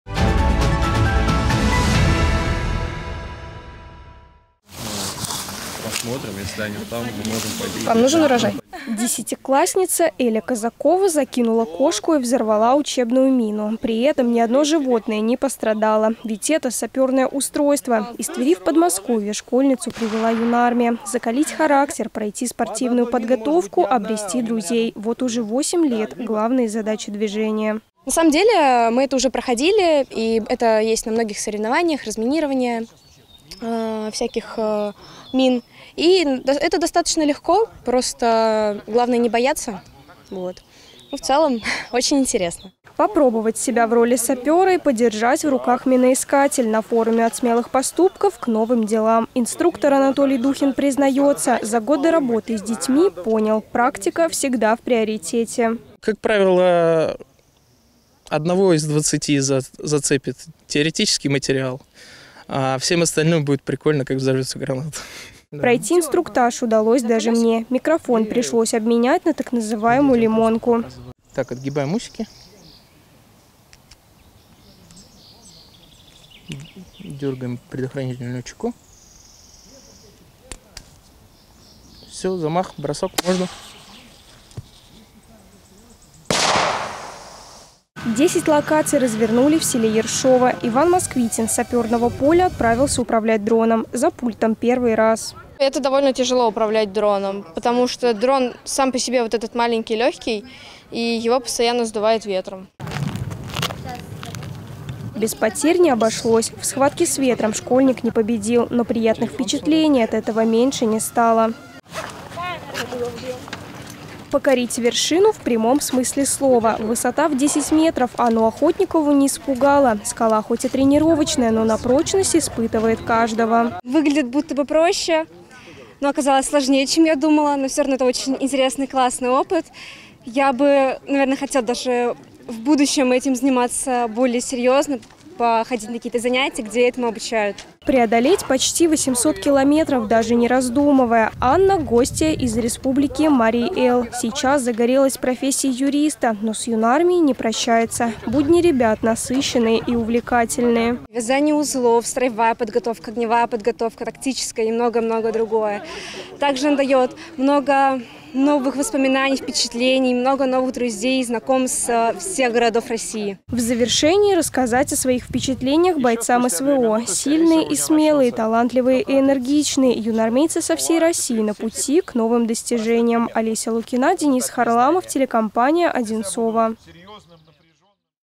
От смелых поступков - к новым делам
От смелых поступков - к новым делам: в Подмосковье стартовал масштабный форум юнармейцев. Ребята со всей России приехали в Ершово, где для них подготовили 10 интересных локаций. Своими впечатлениями от управления дронами, скалолазания и поиска мин, они поделились с Олесей Лукиной.